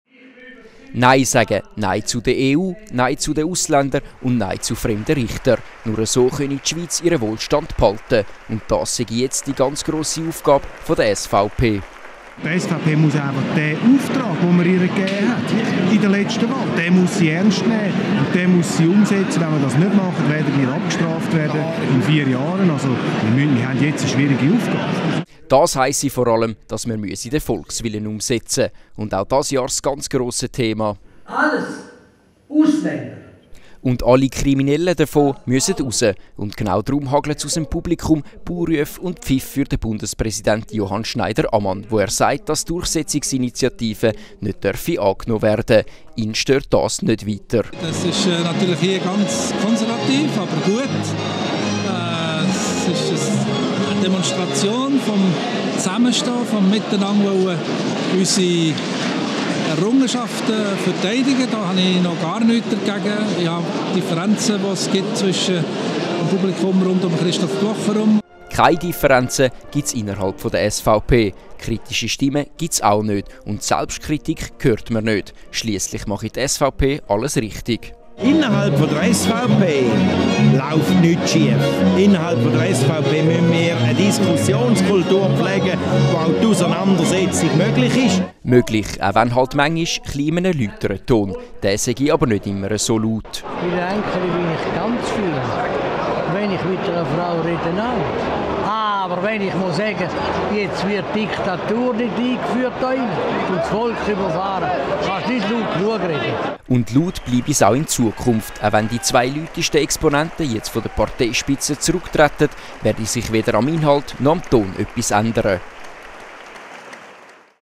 TELE TOP Albisguetli Tagung SVP
Die Zürcher SVP hat ihre traditionelle Albisgüetli-Tagung durchgeführt. Gastredner war Bundespräsident Johann Schneider-Ammann. Zuvor sprach - wie jedes Jahr - Christoph Blocher.